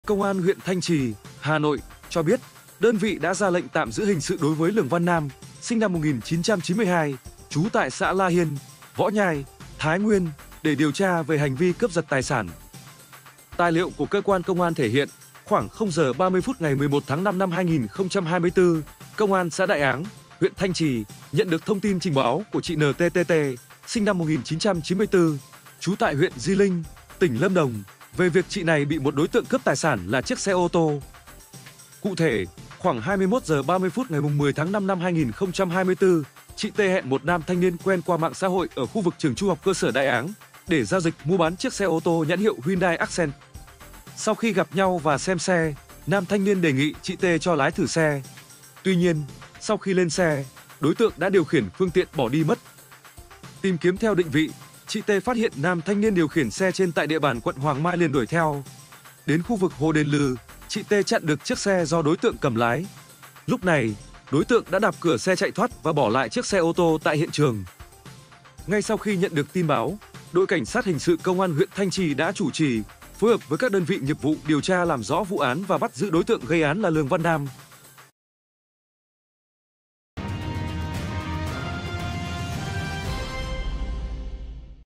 Lời khai của kẻ hỏi mua ô tô, lái thử rồi phóng xe bỏ trốn
Công an huyện Thanh Trì (Hà Nội) cho biết, đơn vị đã ra lệnh tạm giữ hình sự đối với Lường Văn Nam (SN 1992, trú tại xã La Hiên, Võ Nhai, Thái Nguyên) để điều tra về hành vi Cướp giật tài sản.

Tài liệu của cơ quan công an thể hiện, khoảng 00h30 phút ngày 11/5/2024, Công an xã Đại Áng (huyện Thanh Trì) nhận được thông tin trình báo của chị N.T.T.T. (SN 1994, trú tại huyện Di Linh, tỉnh Lâm Đồng) về việc chị này bị một đối tượng cướp tài sản là chiếc xe ô tô.